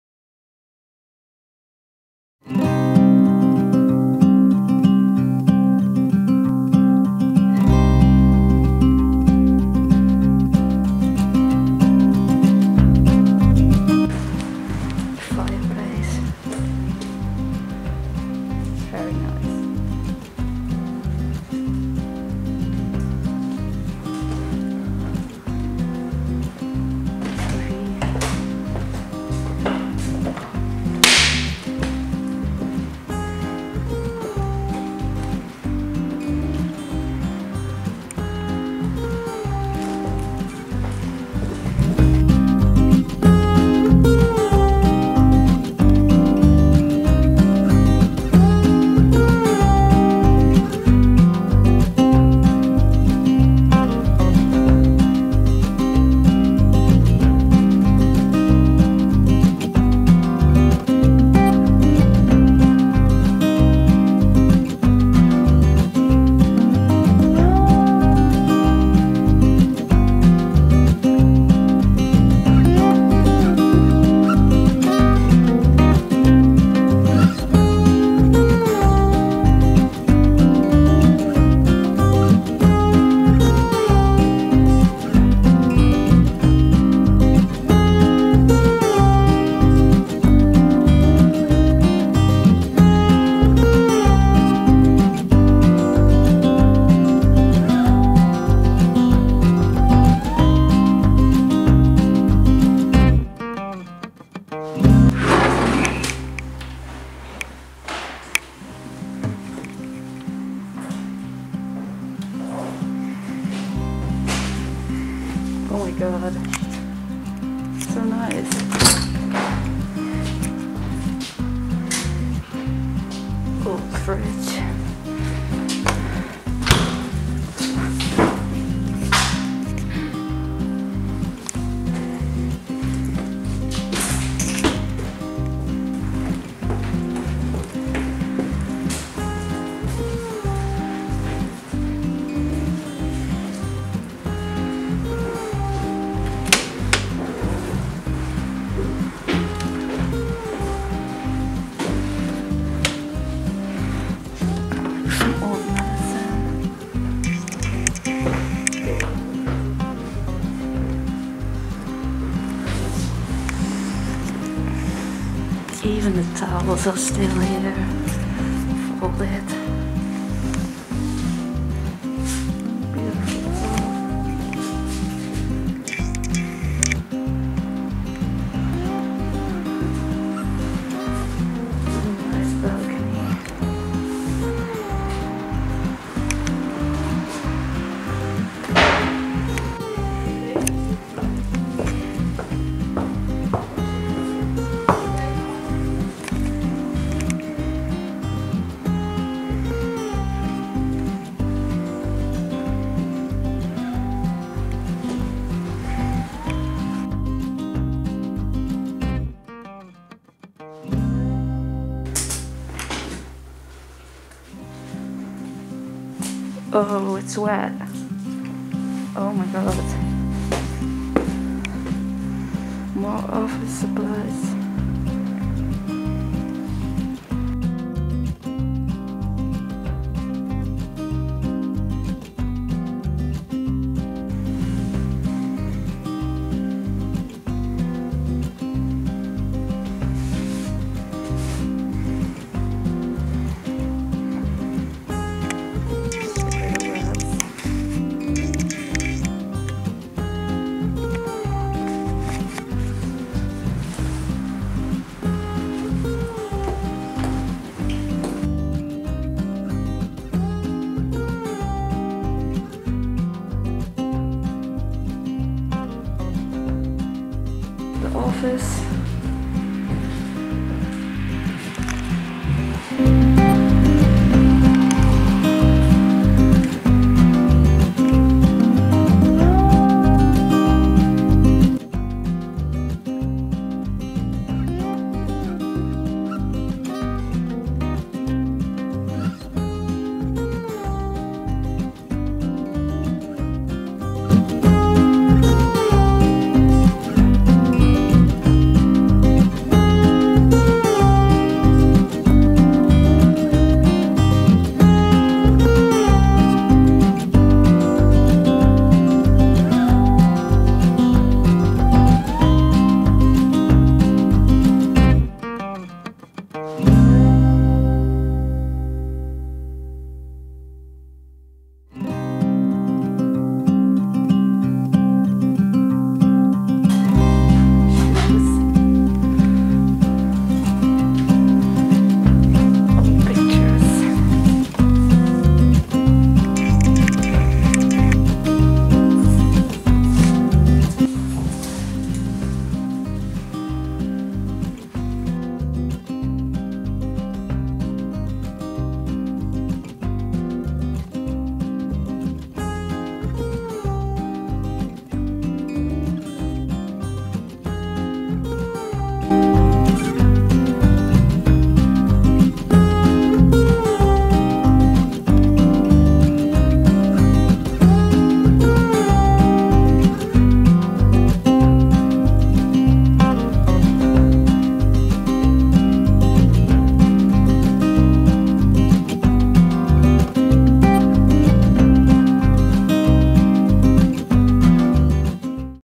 Urbex Abandoned House EVERYTHING IS STILL THERE!
Villa " met de Noorderzon vertrokken " volgens de Urbex map.

I tried to make nice shots but i forgot to turn on the image stabilization, so i am sorry that i shake a little bit.